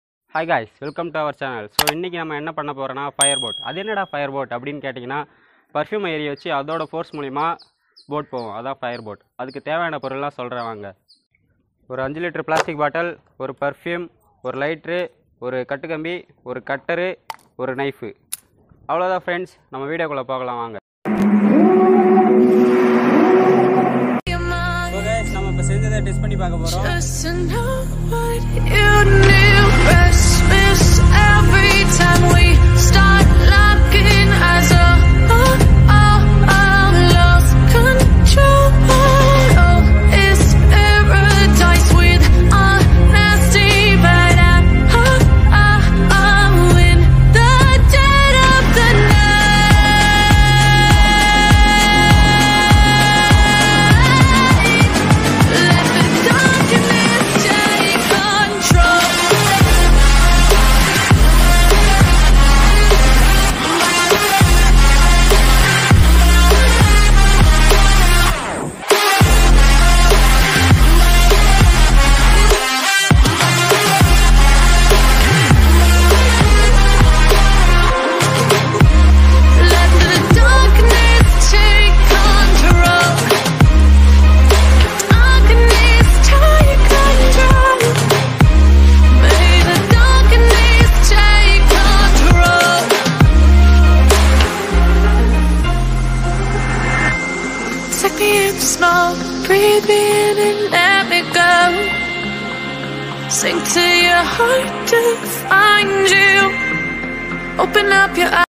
Fire Boat With perfume tamil | Experiment with perfume
experiment with perfume
fire boat

warning:
This experiment done under experts supervision and with full safety and this video is only for demonstration and educational purposes only please don't try to repeat or recreate actions shown in this video